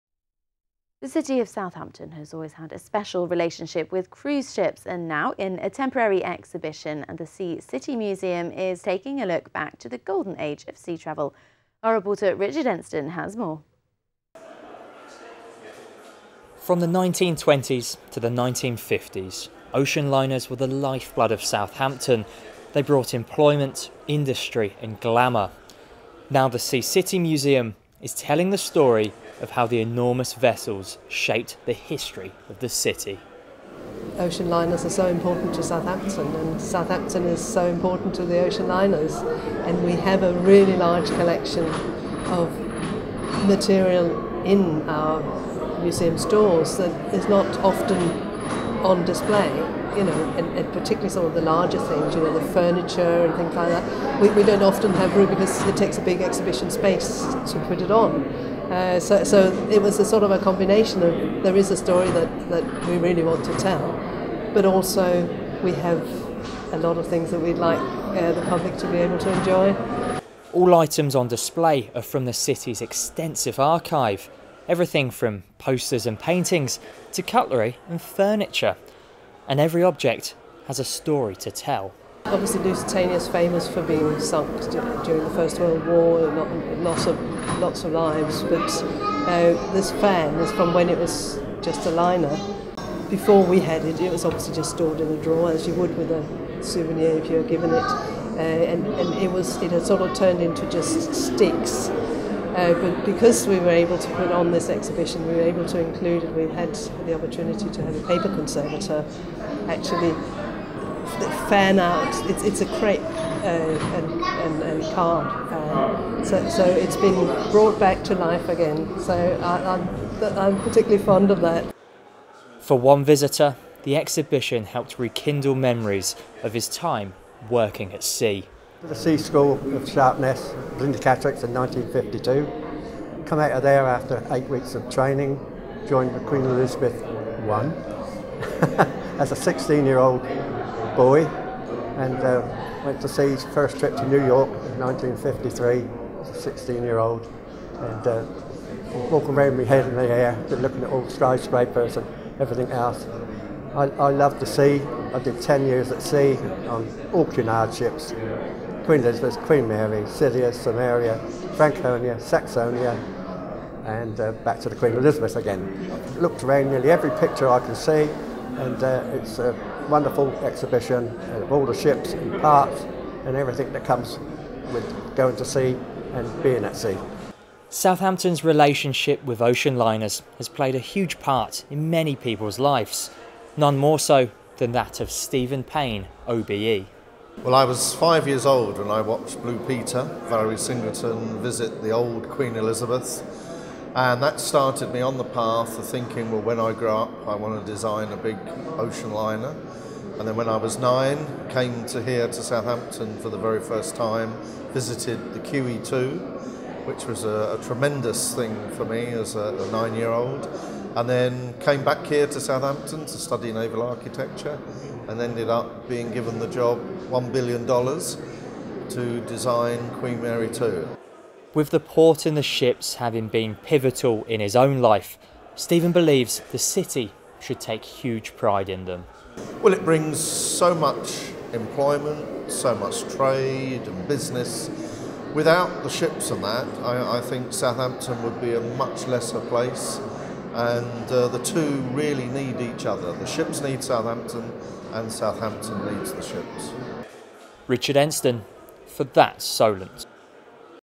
Sea City Museum Pays Homage To Historical Ships
Sea City Museum launches a temporary exhibition to show appreciation to the ships that shaped the history of the city of Southampton. Our reporter Richard Ensten has the details.